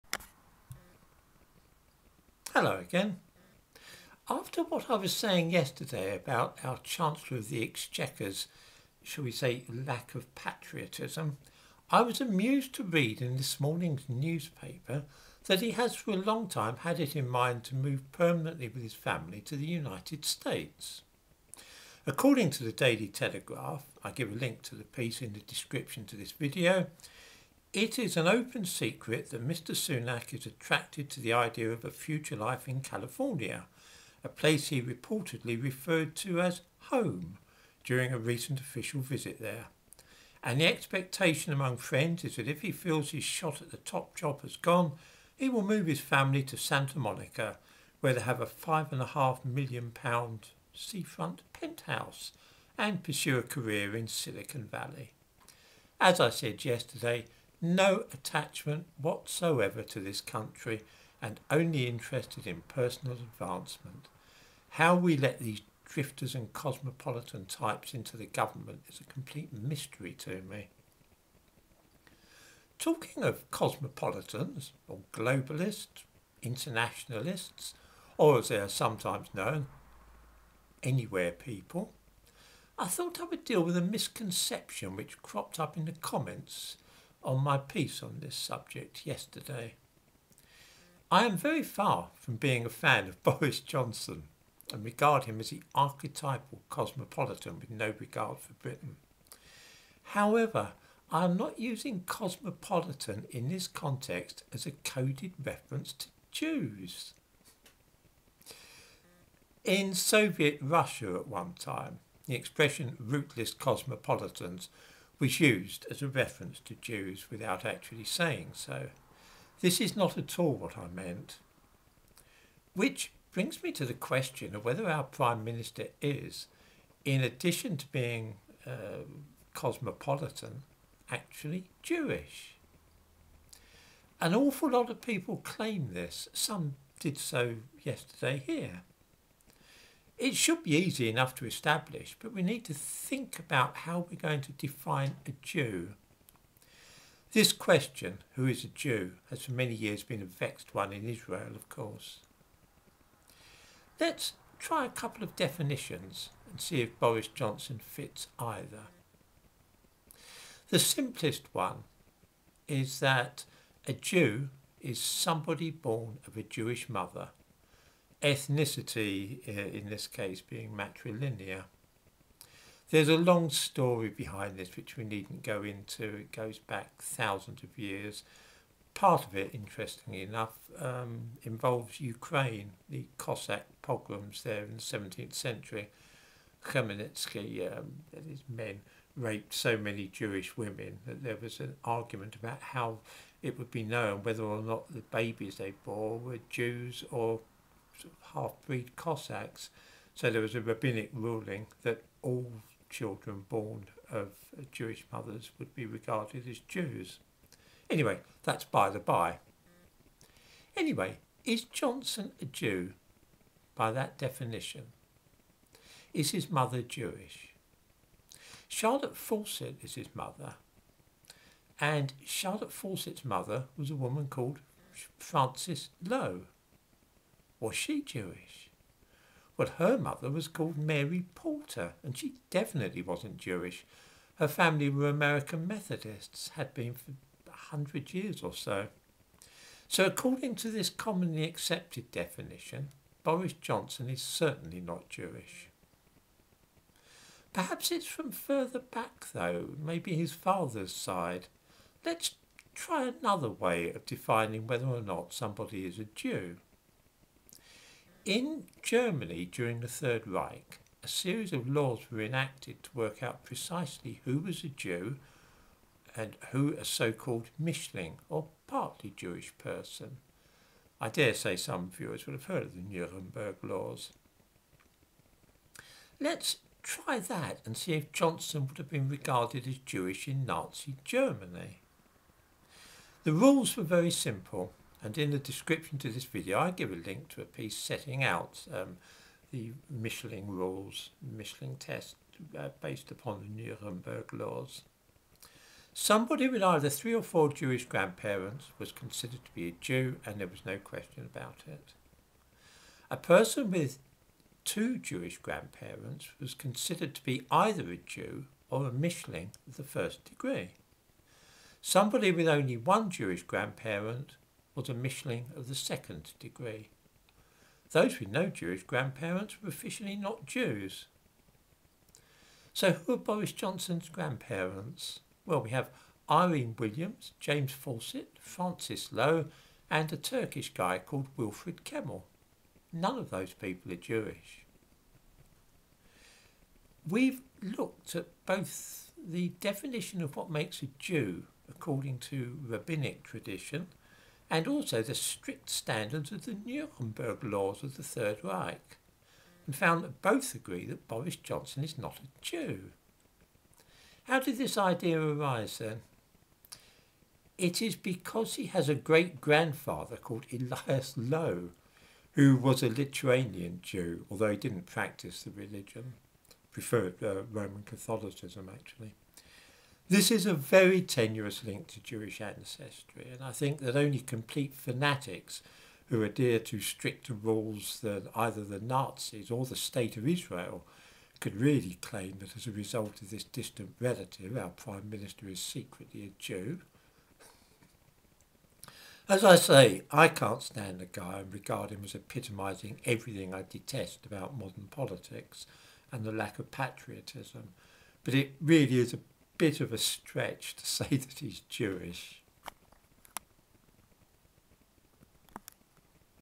Is Boris Johnson Jewish and will Rishi Sunak move to California?
The suggestion that the British cabinet is full of rootless drifters with little attachment to this country should not be taken as an indication that any of them are Jews. We look at one particular and dubious claim.